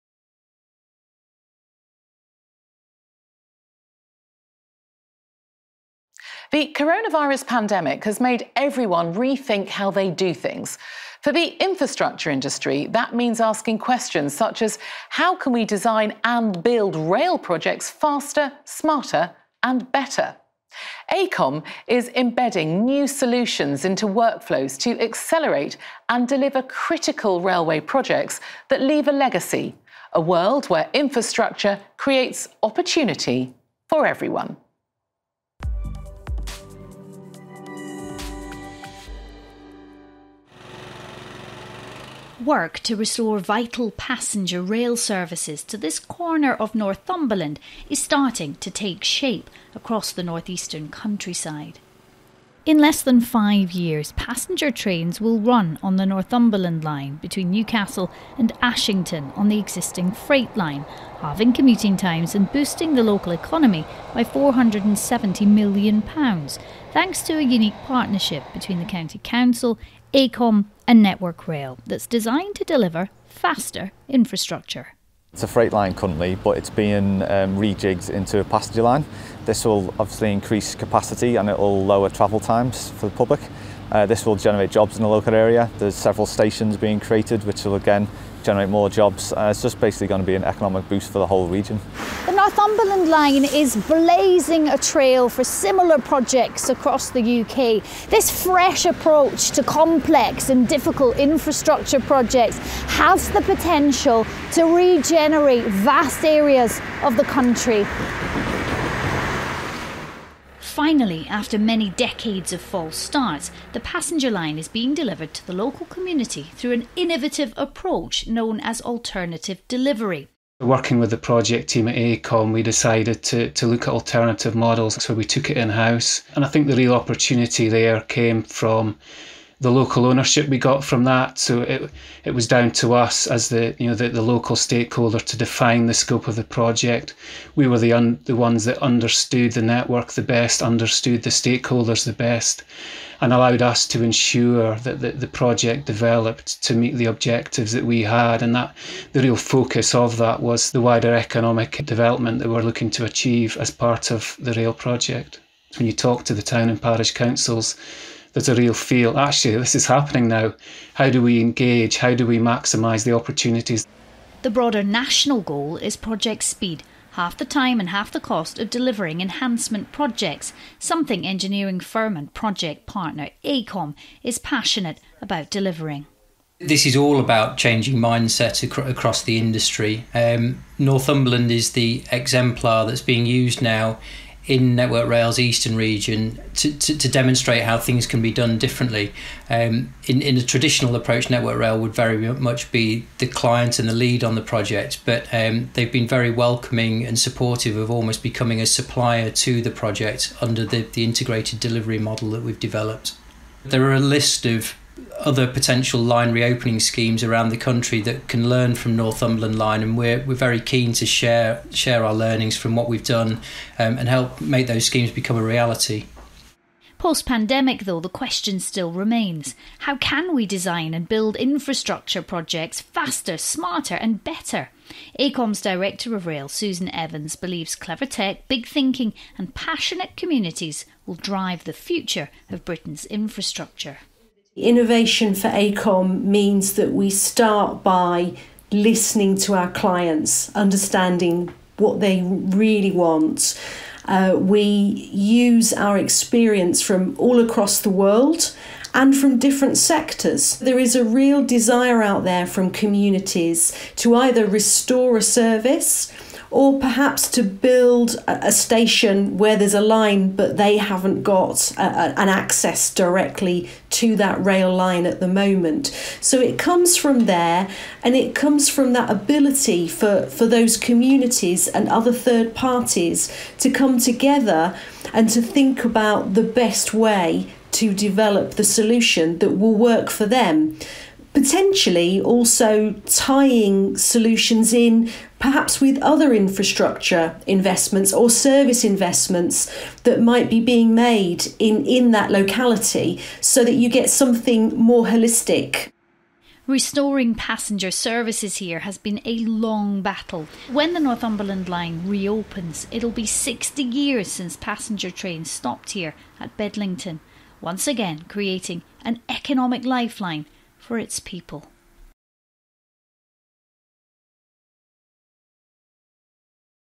Reintroducing Passenger Services | The Northumberland Line Redevelopment
Perfect Circle, appointed via SCAPE Consultancy, has partnered with Network Rail, Northumberland County Council, and SLC Rail to accelerate the redevelopment of the Northumberland Line which will reintroduce passenger rail services to an existing freight-only line by 2024. This film was originally aired at the Rail Industry Innovation Conference.